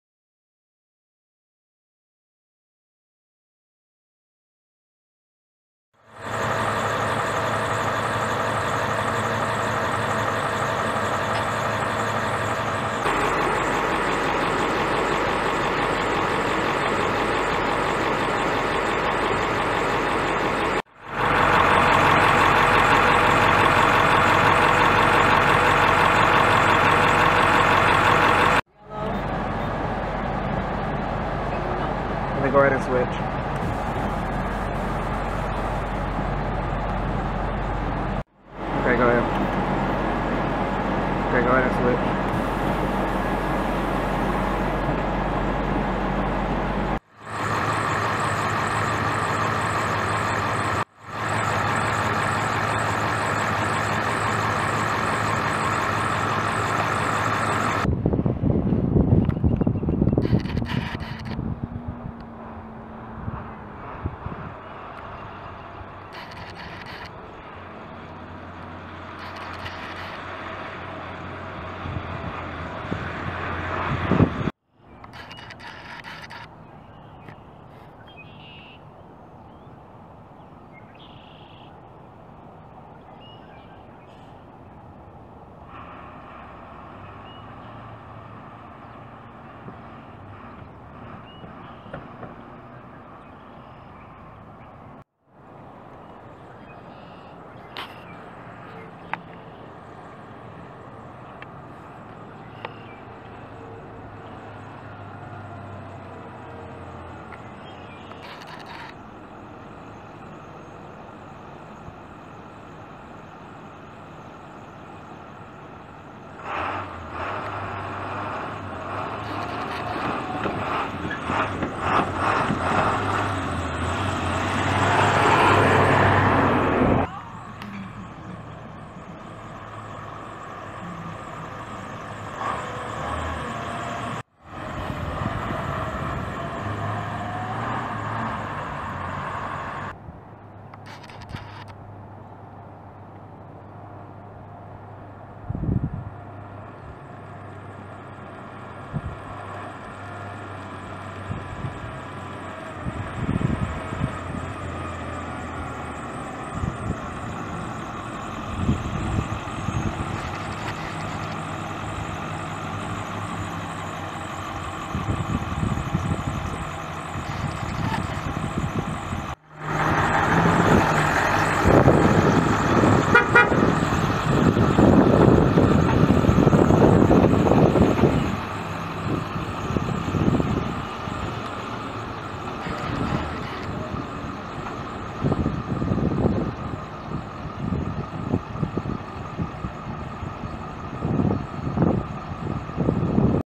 2003 IC CE200: Messing With Bus 03-21
Recorded: March 10, 2018 (03/10/18)
Various clips of bus 03-21.
~03-12 Productions